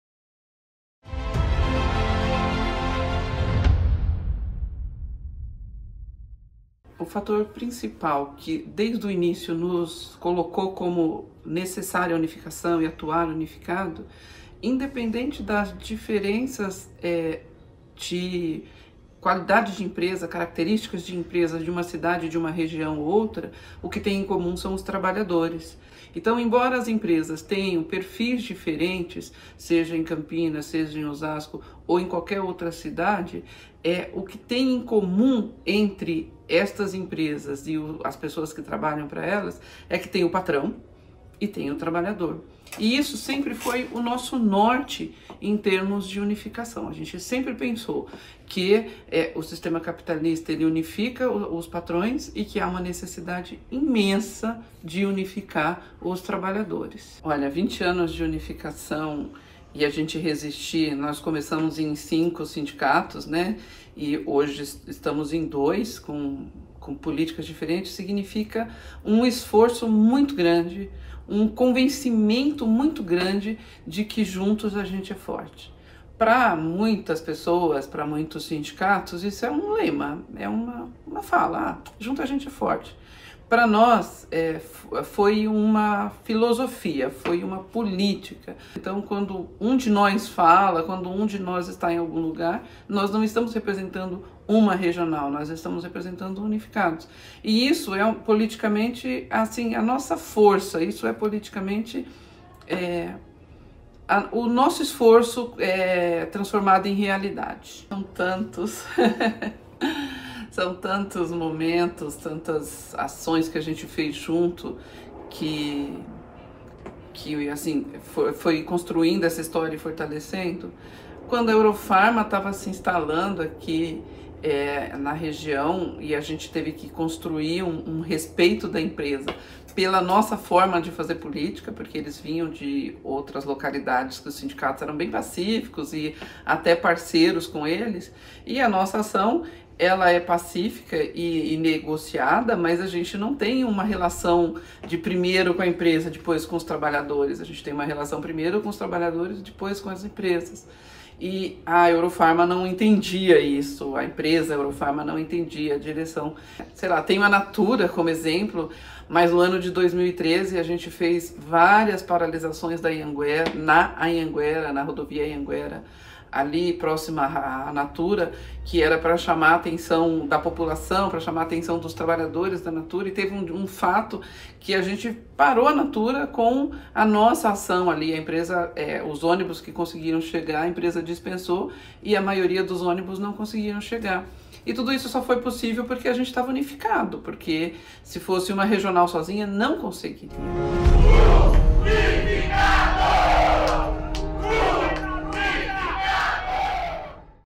20 anos de unificação do Sindicato Químicos Unificados - NILZA
Nilza, dirigente sindical da Regional Osasco, argumenta que o sistema capitalista unifica os patrões com o objetivo de favorecer a burguesia, ou seja, são as empresas que detém os meios de produção e, por isso, é necessário construir a unificação da classe trabalhadora para resistir e somar forças para combater as opressões impostas pelas indústrias. “Nosso esforço transformado em realidade”, diz.

Assista o relato na íntegra sobre os 20 anos de unificação do Sindicato Químicos Unificados!